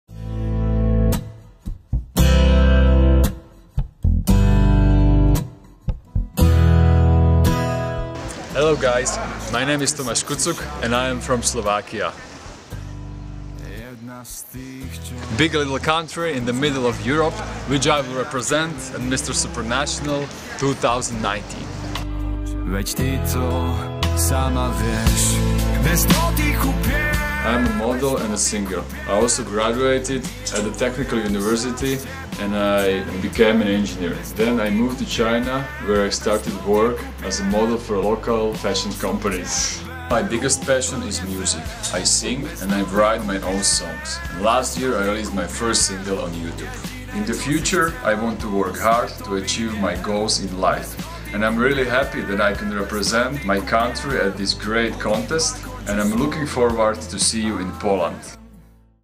Mister Supranational Slovakia 2019 Introduction
Tomas Kucuk is representing Slovakia at Mister Supranational 2019.

Mister Supranational Official channel